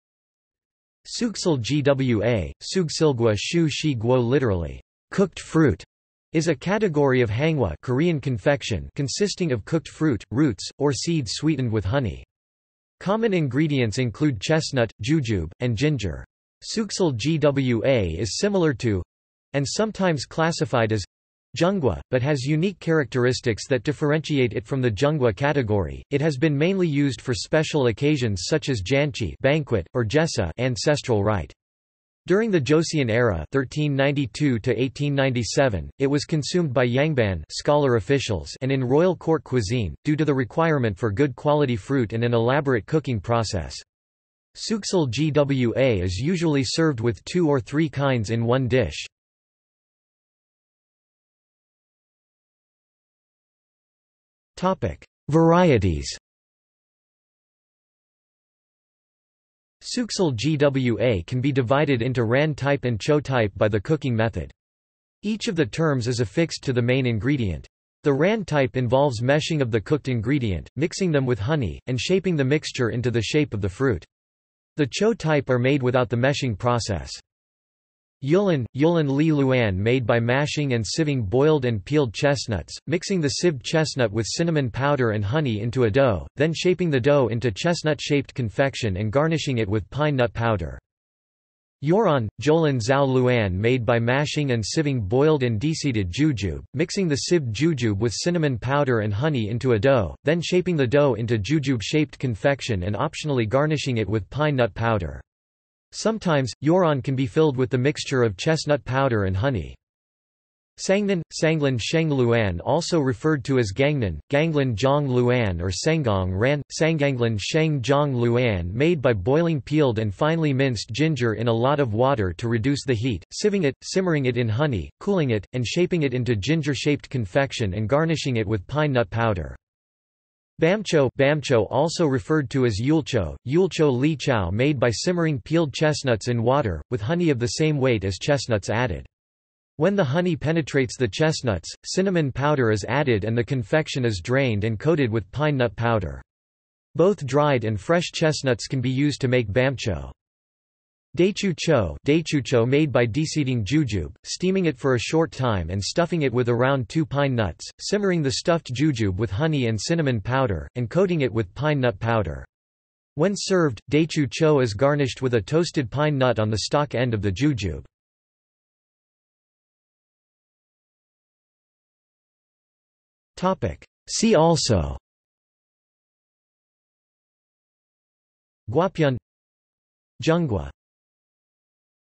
Suksilgwa | Wikipedia audio article
This is an audio version of the Wikipedia Article:
Suksilgwa

00:00:51 undefined
00:02:49 undefined

- Socrates

SUMMARY
Suksil-gwa (숙실과; 熟實果), literally "cooked fruit", is a category of hangwa (Korean confection) consisting of cooked fruit, roots, or seeds sweetened with honey. Common ingredients include chestnut, jujube, and ginger. Suksil-gwa is similar to—and sometimes classified as—jeonggwa, but has unique characteristics that differentiate it from the jeonggwa category.It has been mainly used for special occasions such as janchi (banquet), or jesa (ancestral rite). During the Joseon era (1392–1897), it was consumed by yangban (scholar-officials) and in royal court cuisine, due to the requirement for good quality fruit and an elaborate cooking process. Suksil-gwa is usually served with two or three kinds in one dish.